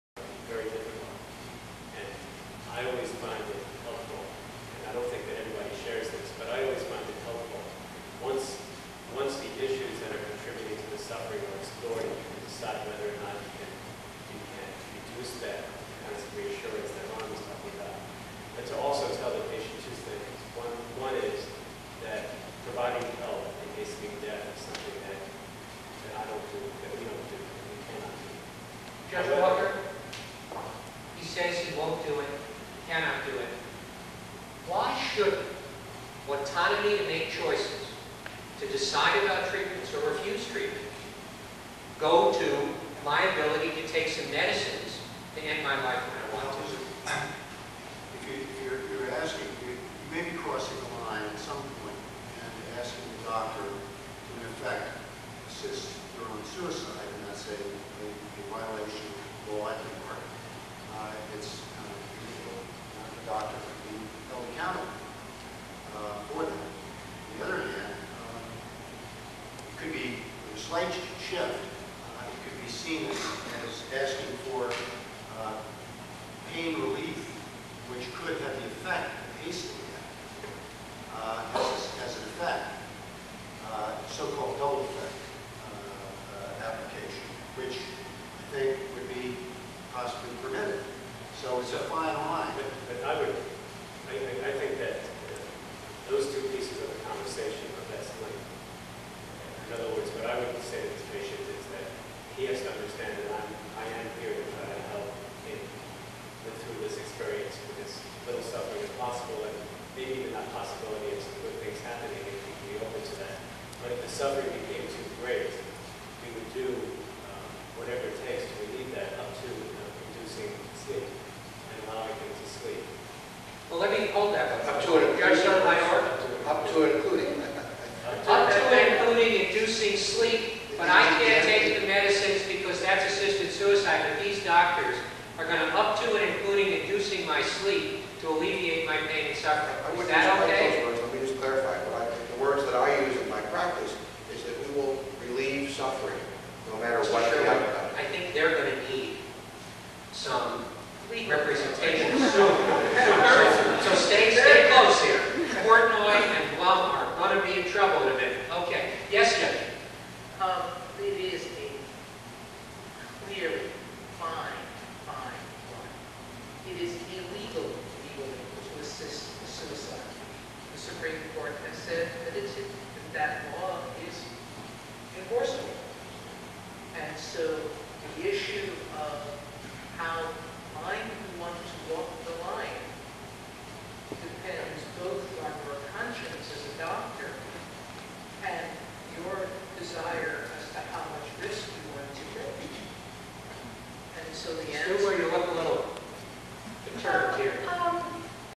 Sotomayor on Assisted Suicide
This video is from September 19, 2000 . Judge Sonia Sotomayor sits on a Federal Bar Council Inns of Court, Panel Discussion on End-of-Life Issues. Her comments come up 2:40 in.